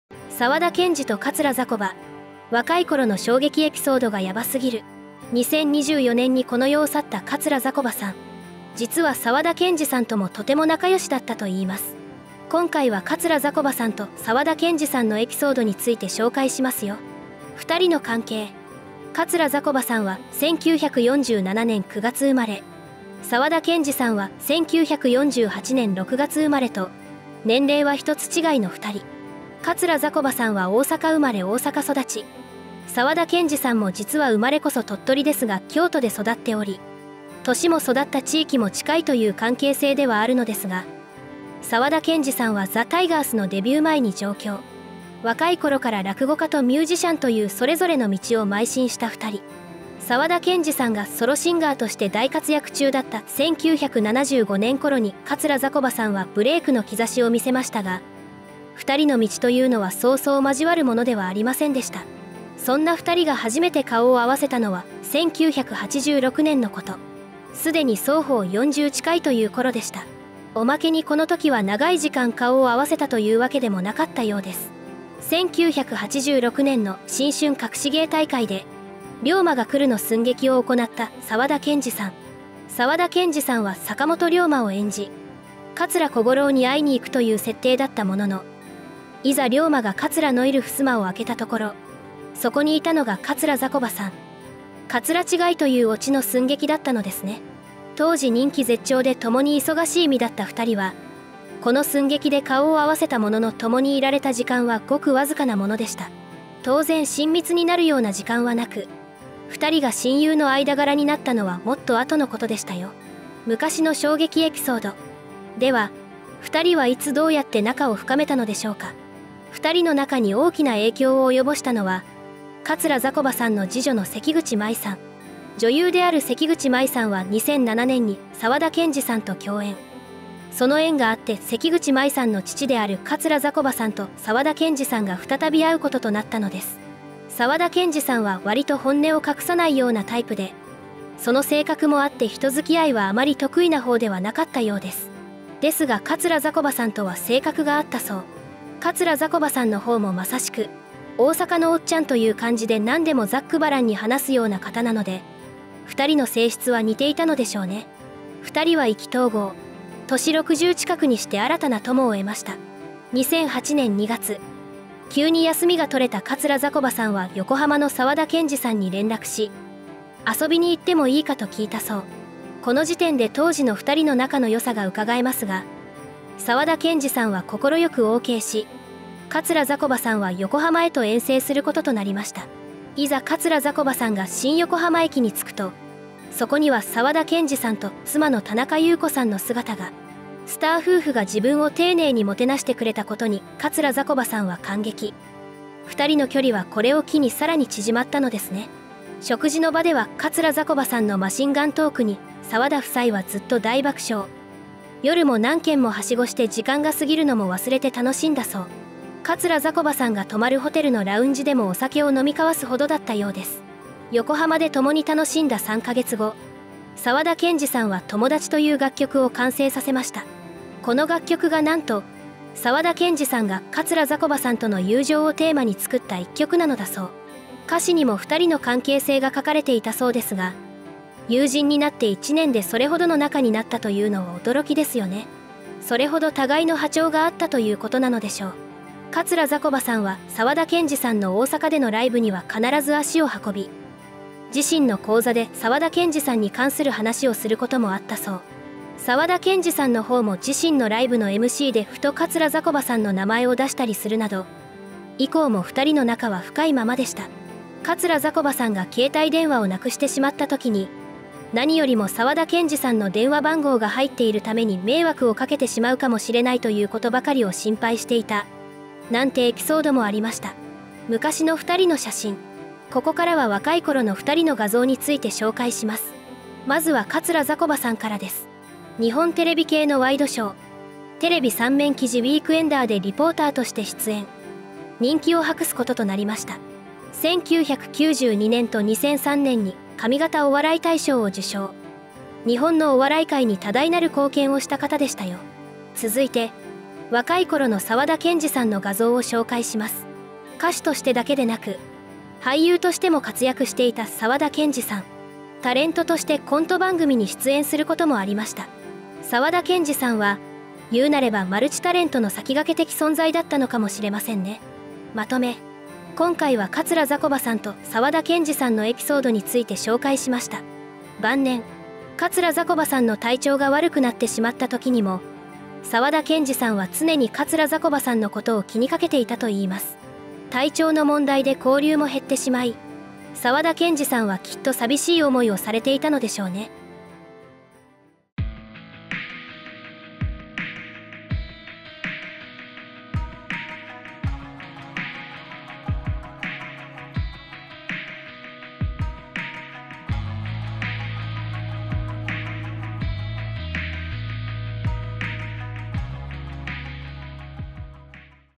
沢田研二と桂ざこば、若い頃の衝撃エピソードがヤバすぎる…「オレが〇〇を隠さなかったから…」
2024年にこの世を去った桂ざこばさん。

実は沢田研二さんともとても仲良しだったといいます。

今回は桂ざこばさんと沢田研二さんのエピソードについて紹介しますよ。